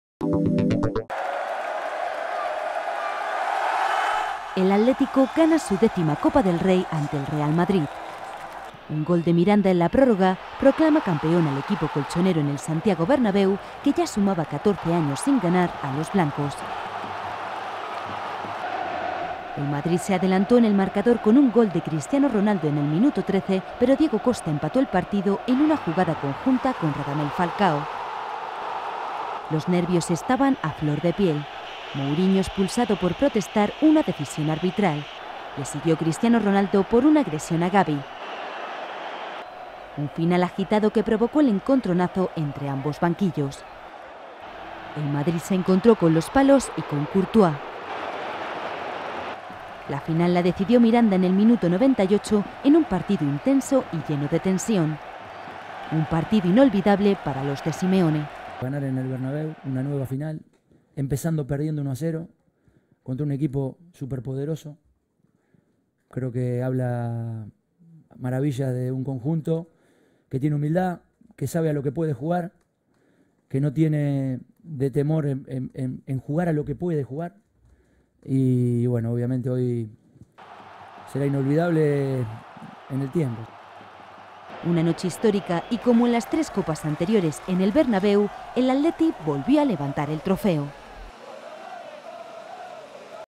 El Atlético logra su Décima Copa del Rey
Madrid, 17 may (EFE).- El Atlético de Madrid saldó cuentas pendientes, con la historia y con su vecino, para conquistar su décima Copa del Rey, la primera después de diecisiete años, tras derribar en la prórroga al Real Madrid (1-2).